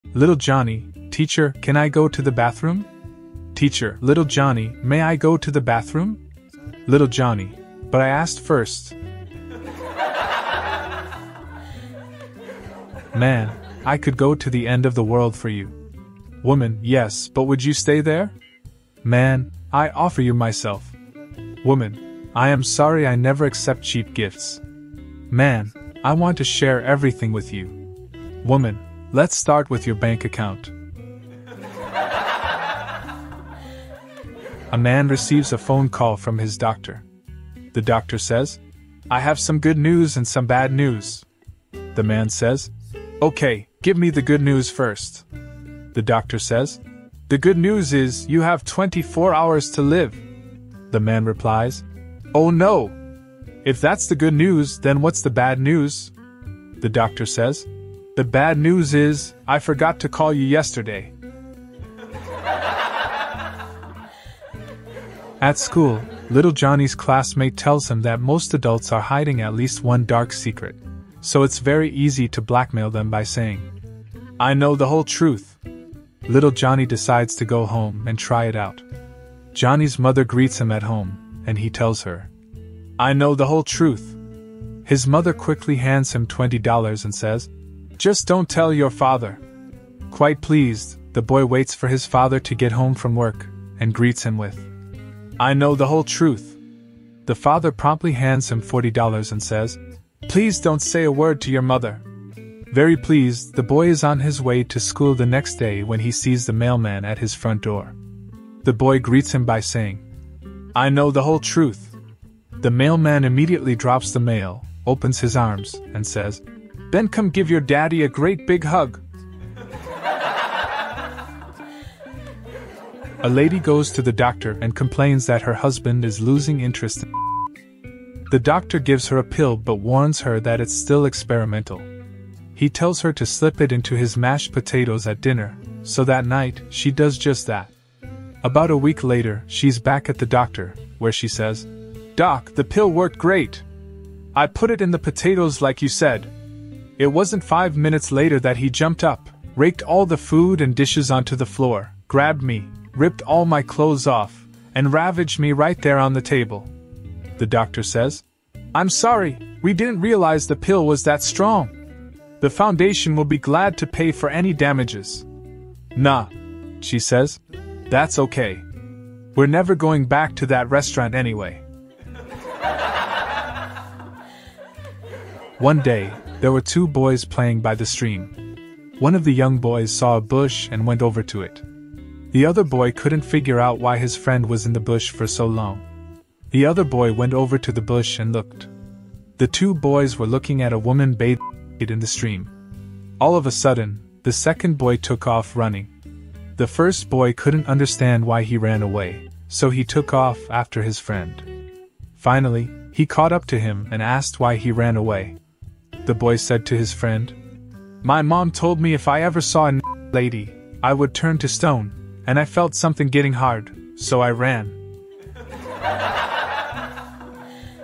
🤣Best Jokes | Two boys were looking at a woman into the loljokes
Two boys were looking at a woman bathing

00:03  Little Johnny: Teacher,
00:13   Man: I could go to the
00:34  A man receives a phone
01:05   At school, Little Johnny's
02:03   A lady goes to the doctor
02:55  One day, there were two